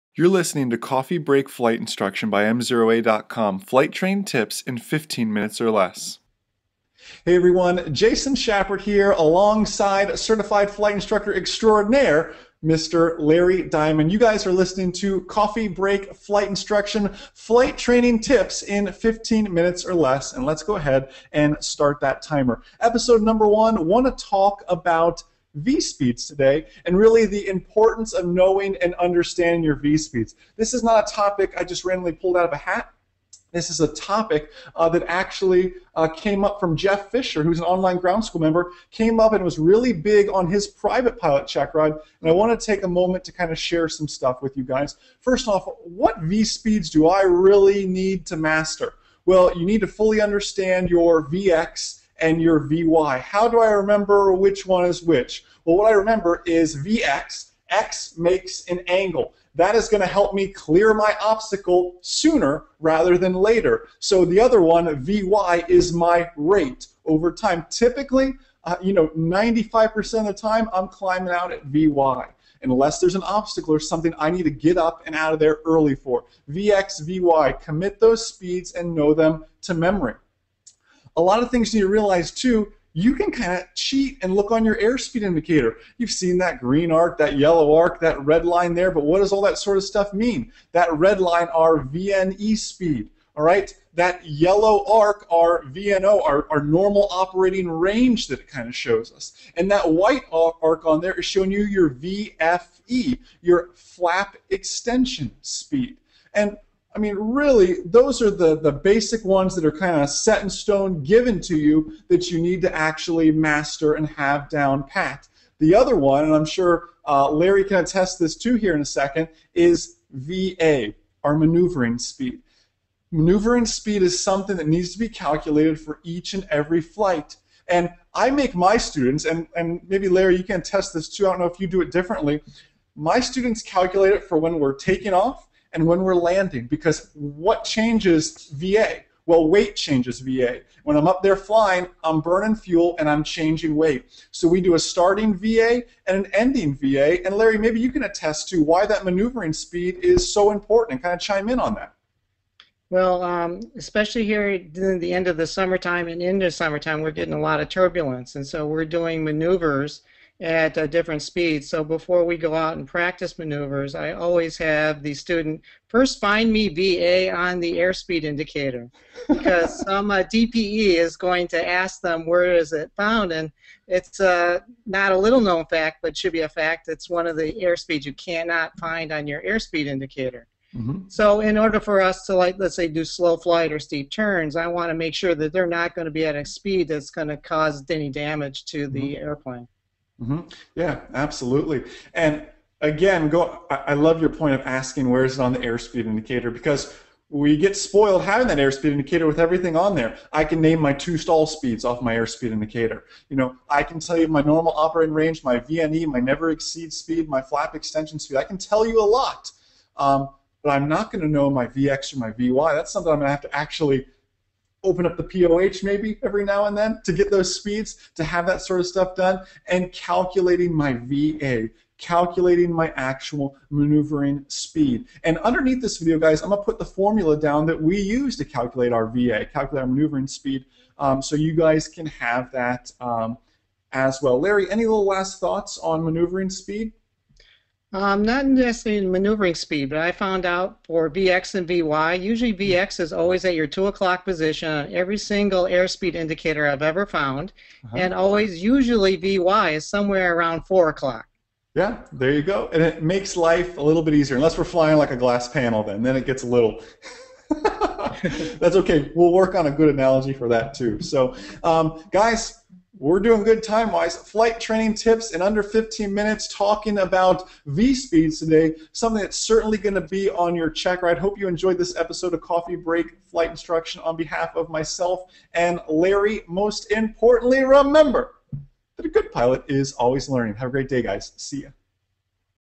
Know Your V Speeds - Coffee Break Flight Instruction by MzeroA
In the inaugural episode of Coffee Break Flight Instruction Jason and Larry talk about the importance of knowing your V speeds.